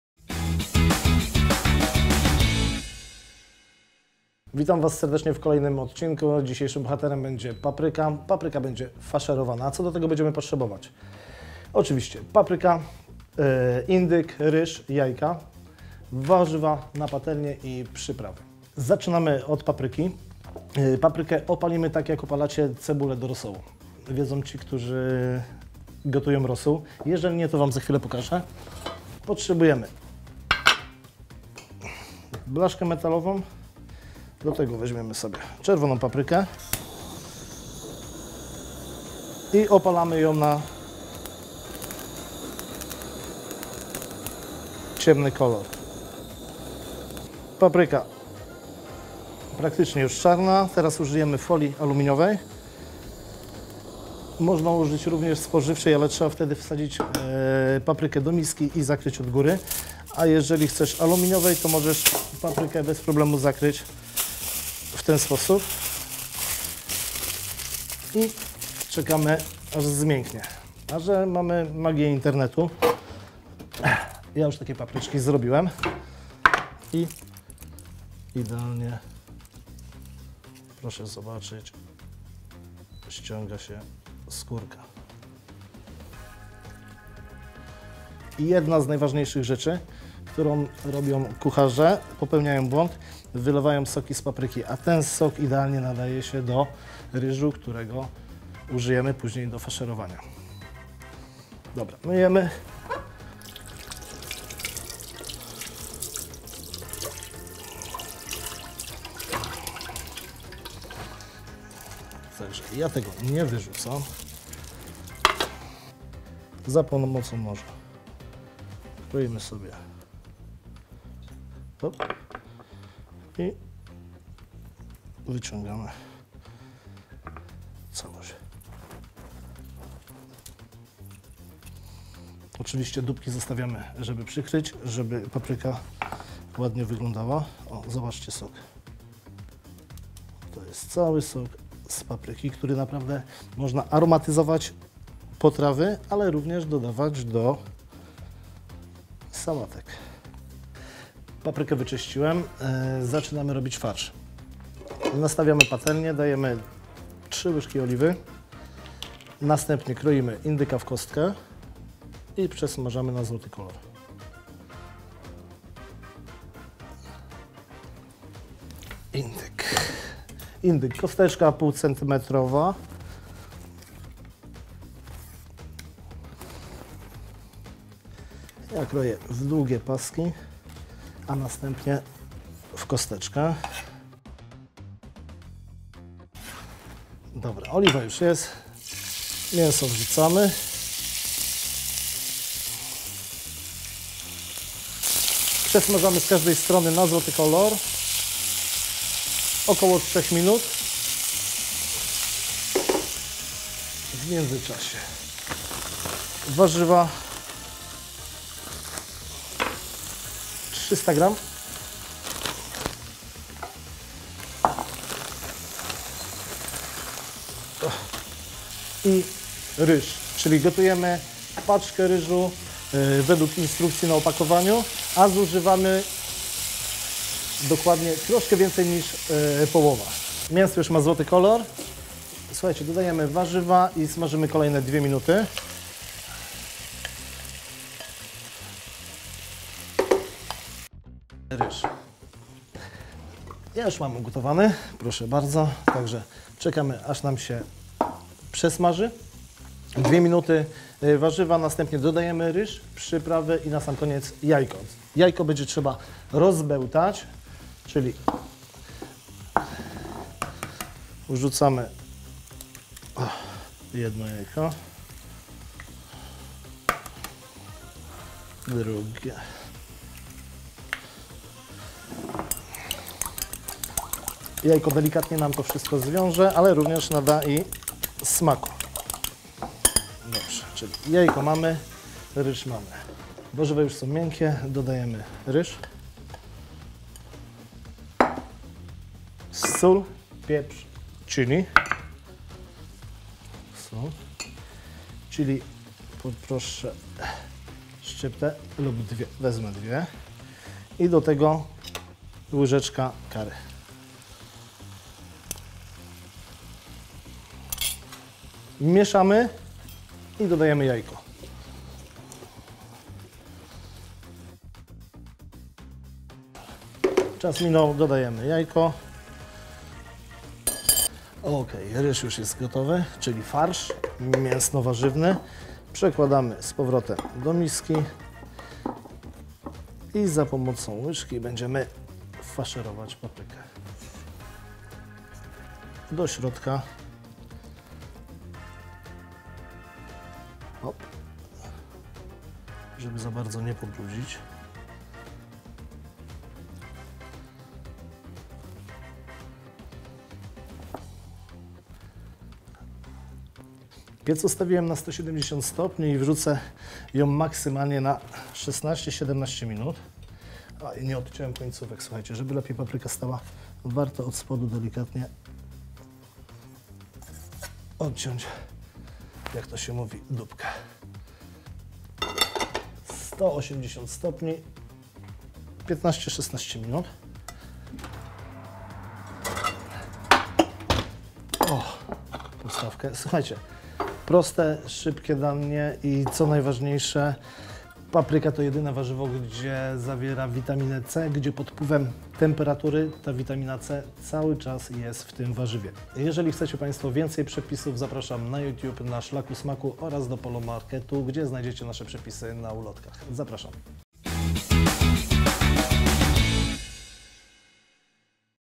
PAPRYKA FASZEROWANA 🥗 🥗 | Na szlaku smaku z POLOmarketem
SKŁADNIKI:
• 4 papryki
• 200 g mięsa z indyka
• 100 g ryżu Marysia
• 300 g mieszanki meksykańskiej Go Tove
• 2 jajka
• 3 łyżki oliwy Philosophie
• 2 szczypty chili
• 1 łyżeczka curry Na Szlaku Smaku
• szczypta soli
• szczypta pieprzu Na Szlaku Smaku
SPOSÓB PRZYGOTOWANIA:
Papryki opal na gazie lub palniku. Gdy warzywa będą miały ciemny kolor, zawiń je w folię aluminiową (pozostaw ok. 10 min). Po czasie „sok” z folii przelej do miski, a paprykę obierz ze skóry, odetnij wieczko (pozbądź się gniazd nasiennych i „środka”). Na patelni rozgrzej tłuszcz, przesmaż pokrojonego w 1 cm kawałki indyka na złoty kolor. Po ok 3 min dorzuć warzywa meksykańskie, a po kolejnych dwóch ryż (ugotowany zgodnie z instrukcją na opakowaniu). Całość delikatnie mieszaj, w międzyczasie przypraw i wbij jajko oraz wlej sok z papryki. Uzyskaną masą nafaszeruj papryki i wstaw do nastawionego na 180 stopni piekarnika. Zapiekaj 16 – 17 minut.